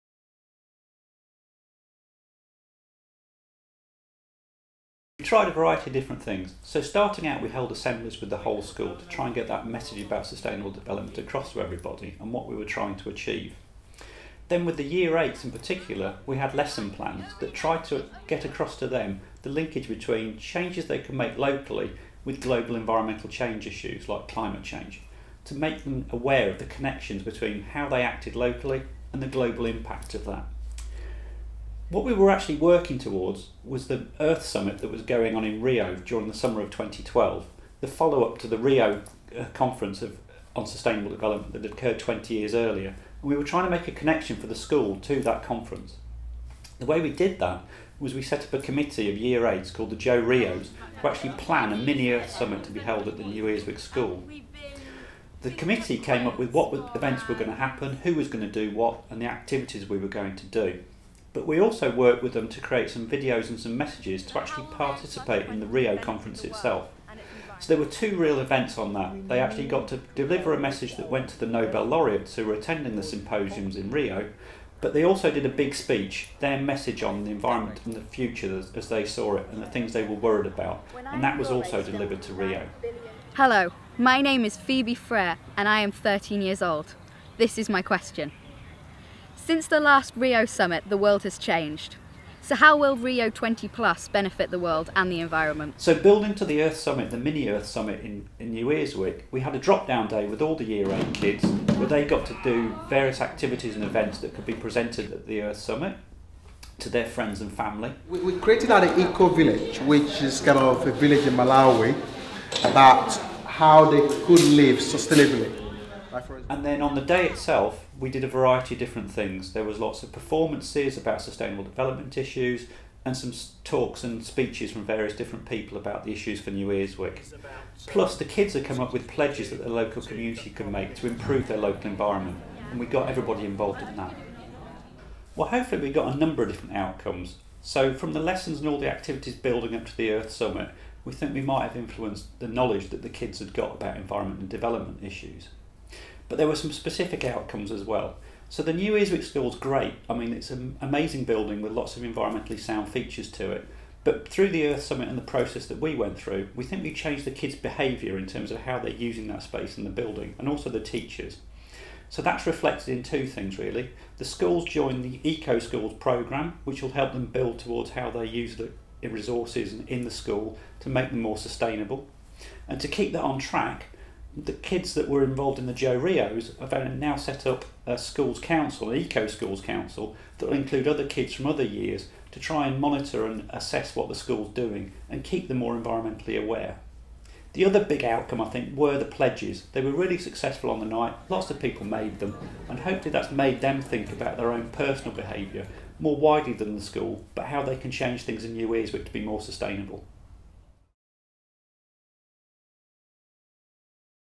The Good Life Initiative's JoRio project
Steve Cinderby explains SEI's engagement with New Earswick Joseph Rowntree School during JoRio project.

Video produced by:
Howard Cambridge and Radek Rudnicki